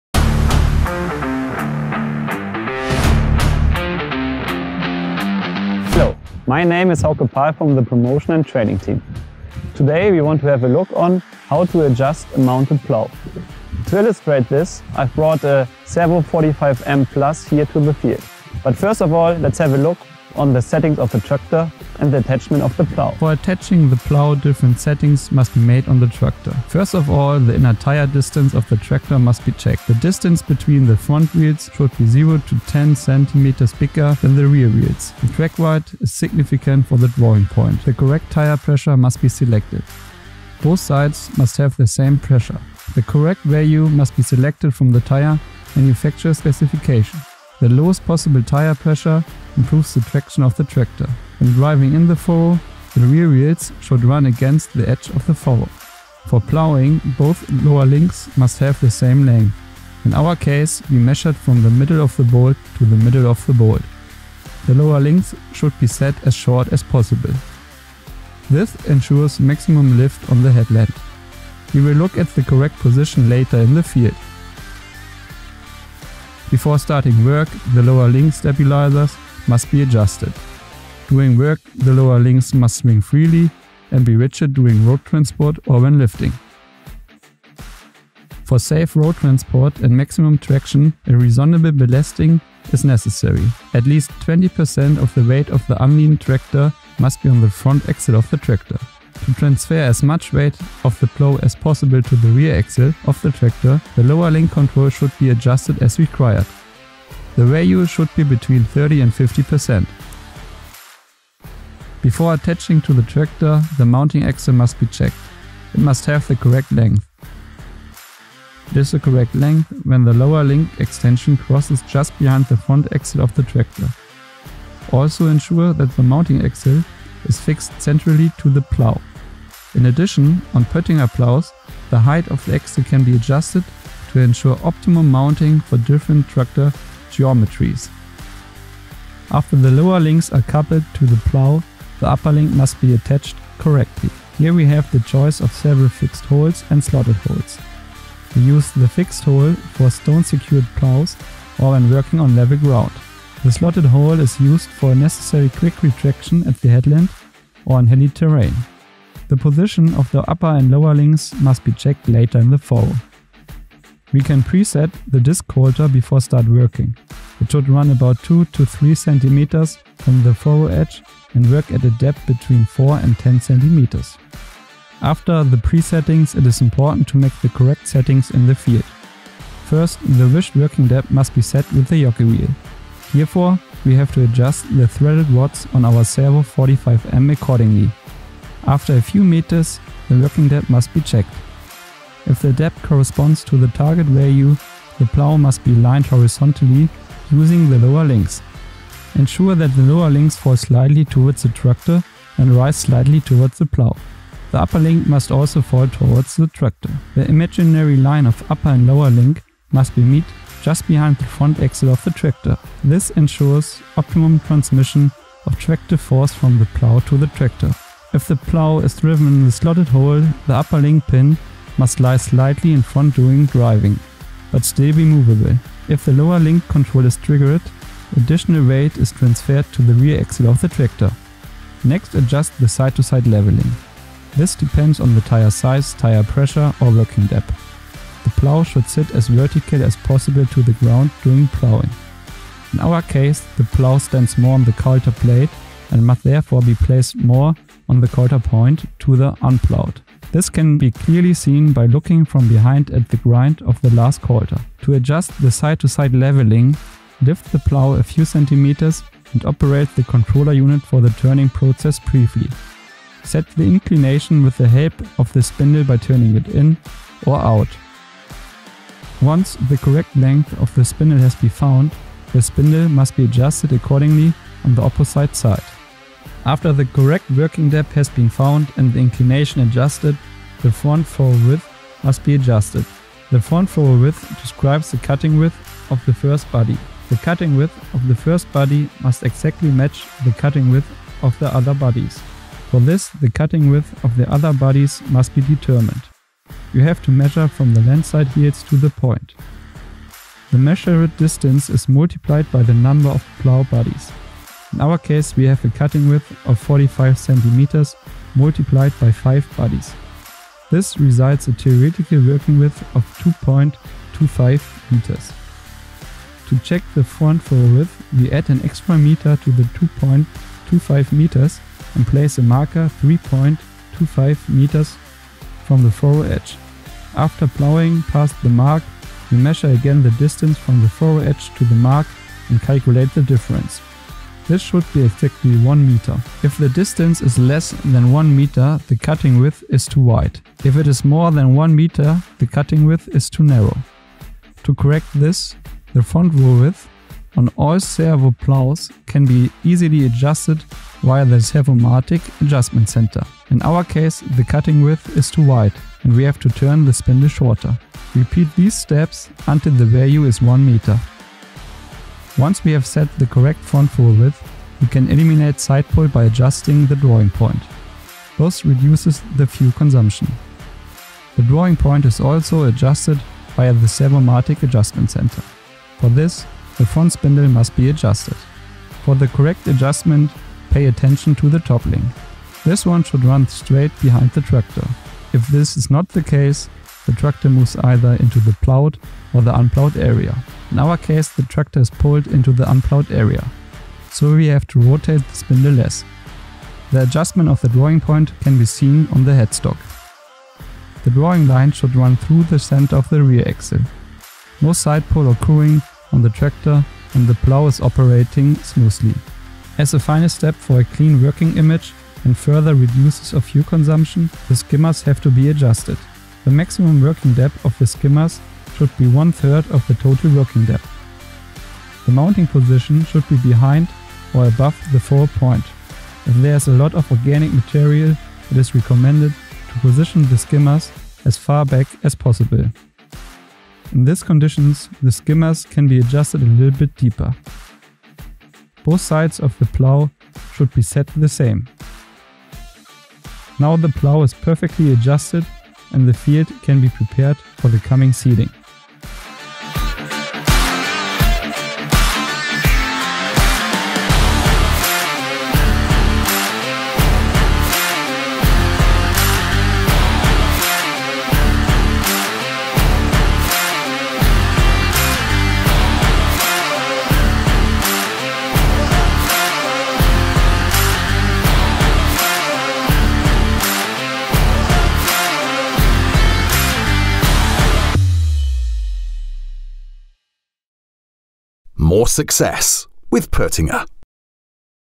Настройка на плугове PÖTTINGER SERVO
Вижте детайлна демонстрация стъпка по стъпка на настройката на плуг PÖTTINGER SERVO 45 M PLUS, която ще гарантира равномерна обработка на почвата и ниско износване на плужните тела.